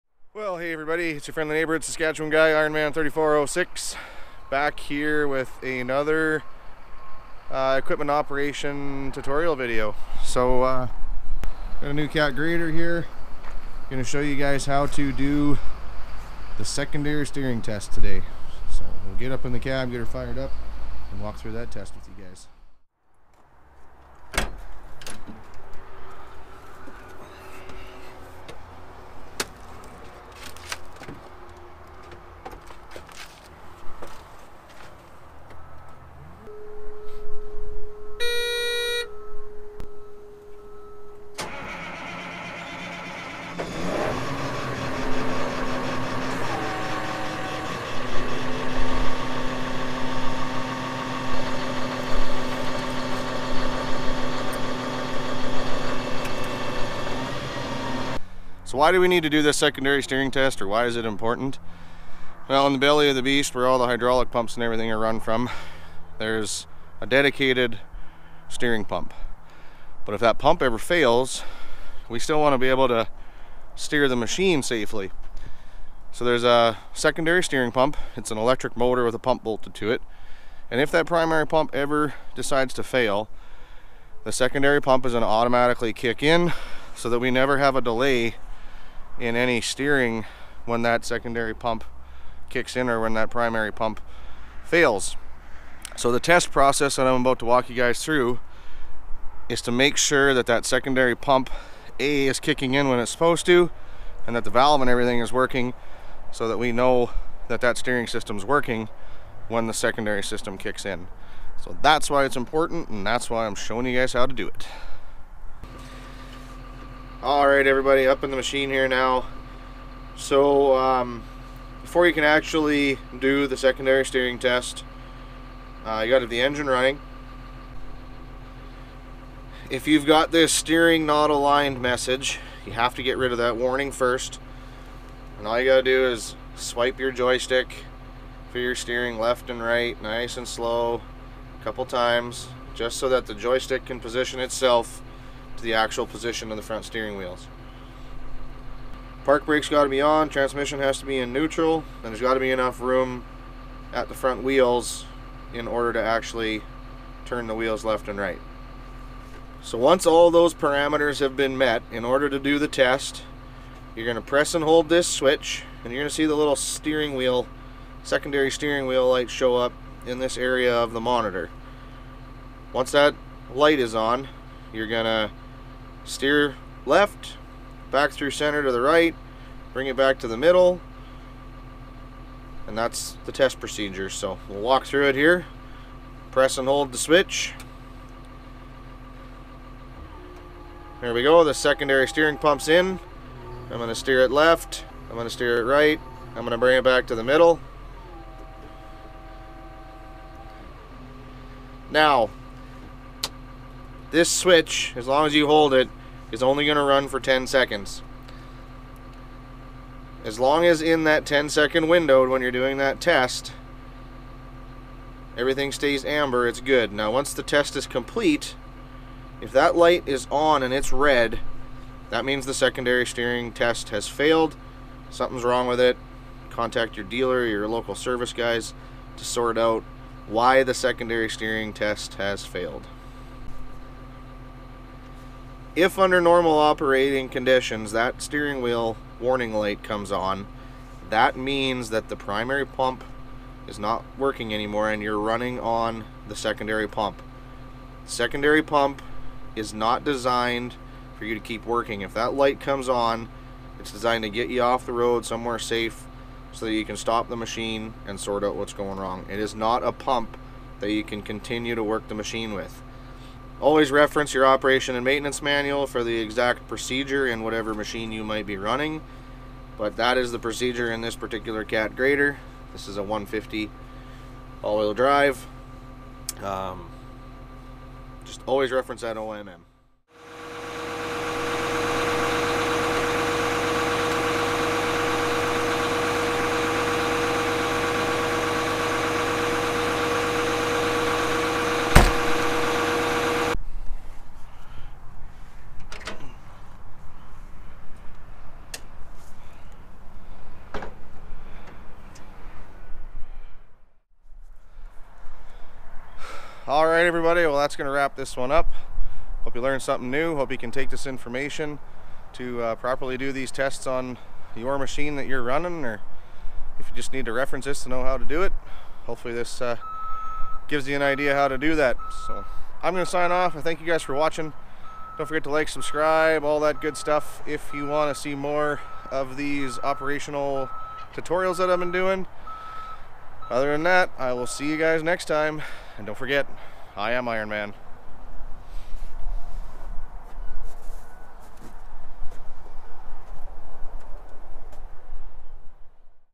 Caterpillar 150 Motor Grader - Secondary Steering Test Tutorial
Welcome back to the channel.  In this operator tutorial video I am going to show you how to do the secondary steering test on a Cat 150 AWD motor grader.  This is a quick and easy test that you should be doing regularly.  I hope this video helps you learn how to do the test and is a future reference if you ever need it. Always reference the OMM for the machine that you are performing the test on to make sure of the proper procedures.